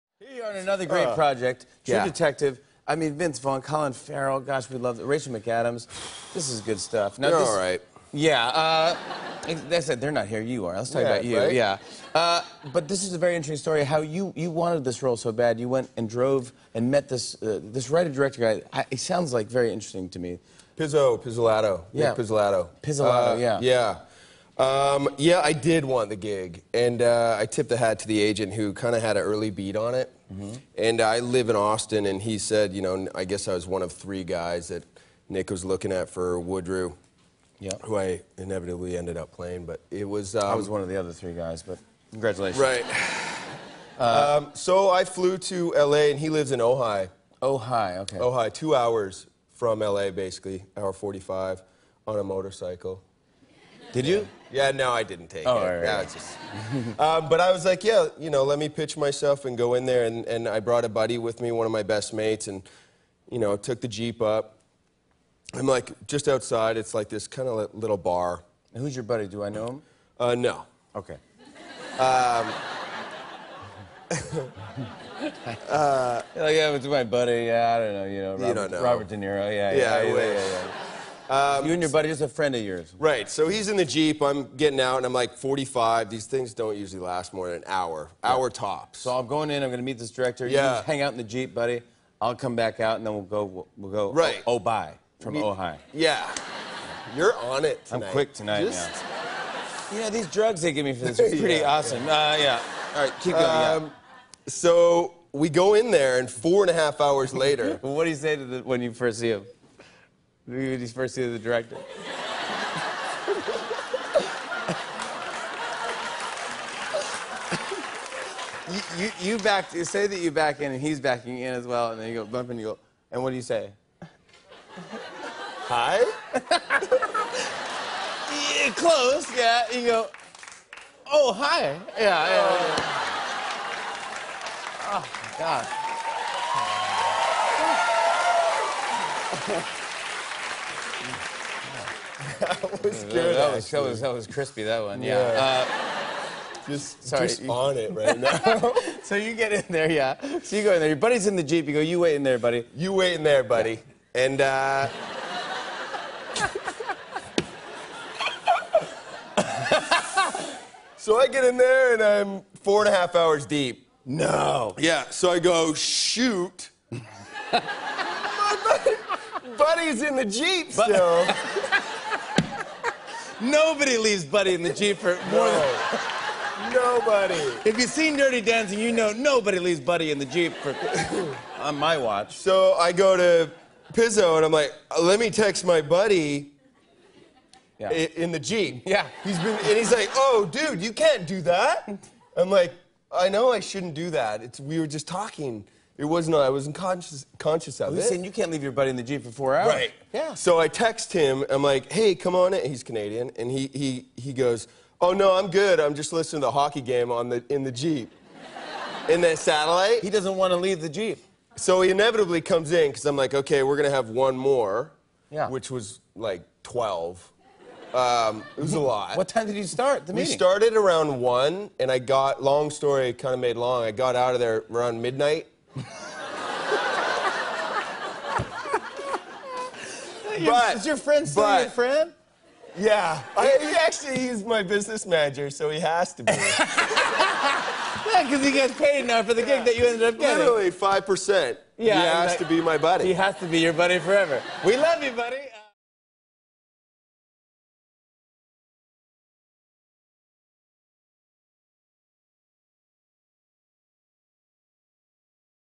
Taylor Kitsch Left a Friend Stranded for True Detective
Taylor Kitsch hitched a ride with a friend to his True Detective audition but ended up forgetting about him for nearly 12 hours.